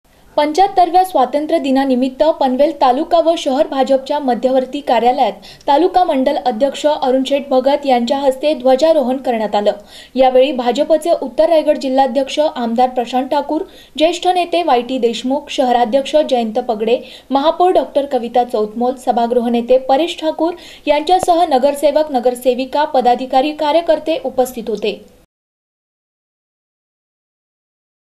पनवेल भाजप कार्यालयात ध्वजारोहण !
७५ व्या स्वातंत्र्य दिनानिमित्त पनवेल तालुका व शहर भाजपच्या मध्यवर्ती कार्यालयात तालुका मंडल अध्यक्ष अरुणशेठ भगत यांच्या हस्ते ध्वजारोहण करण्यात आले. यावेळी भाजपचे उत्तर रायगड जिल्हाध्यक्ष आमदार प्रशांत ठाकूर, ज्येष्ठ नेते वाय. टी. देशमुख, शहराध्यक्ष जयंत पगडे, महापौर डॉ. कविता चौतमोल, सभागृहनेते परेश ठाकूर, यांच्यासह नगरसेवक, नगरसेविका, पदाधिकारी, कार्यकर्ते उपस्थित होते.